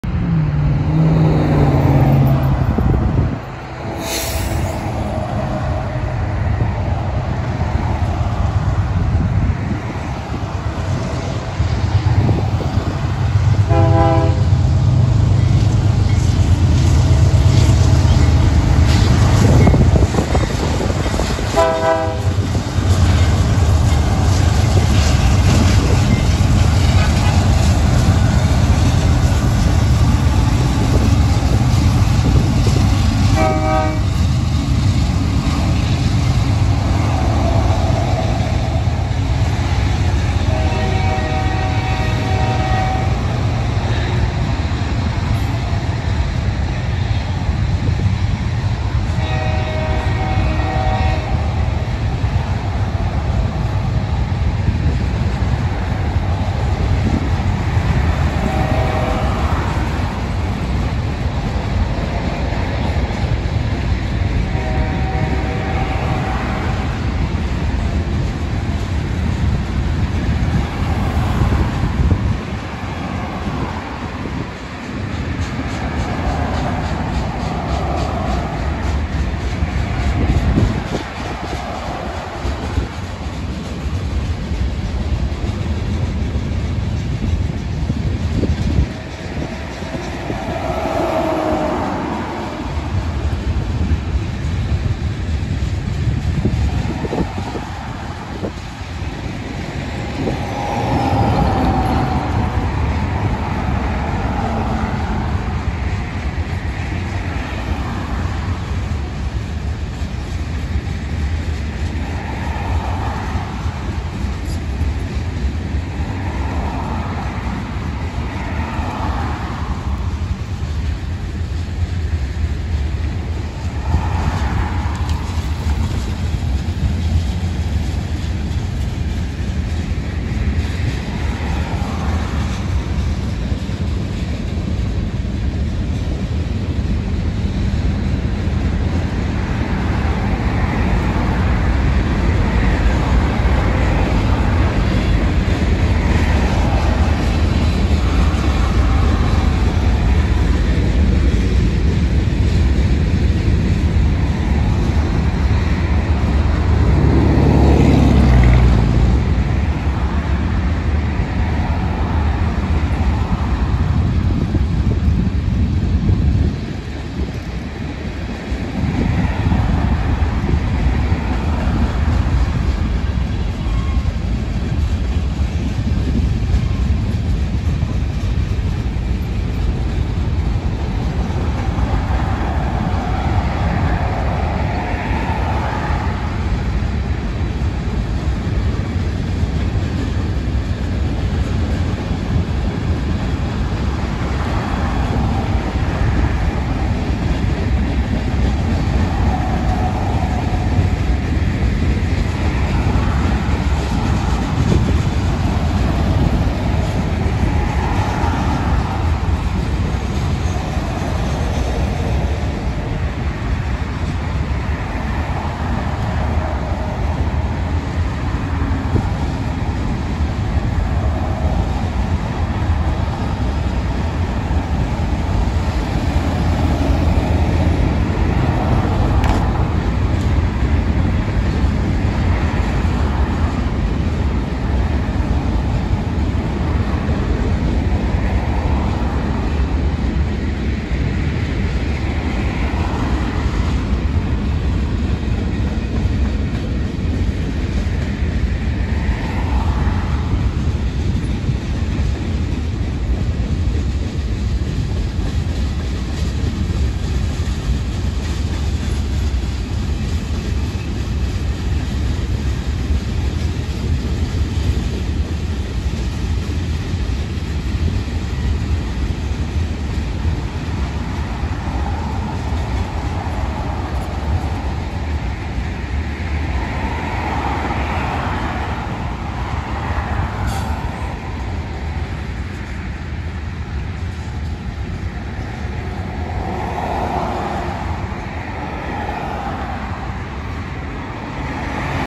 UP 5565 leads a Manifest Train at Roanoke, TX
3/2/3 at Roanoke at 11:36 AM the first Train I saw while I was out Railfaning was a Union Pacific Manifest Train heading South down the Union Pacific Choctaw Subdivision with 4 Engines all on Point.

Engines were

UP 5565 GE AC44CWCTE
UP 7232 GE AC44CW
UP 2745 GE ES44AC-H (Tier 4 Credit)
UP 2733 GE ET44AH (Tier 4)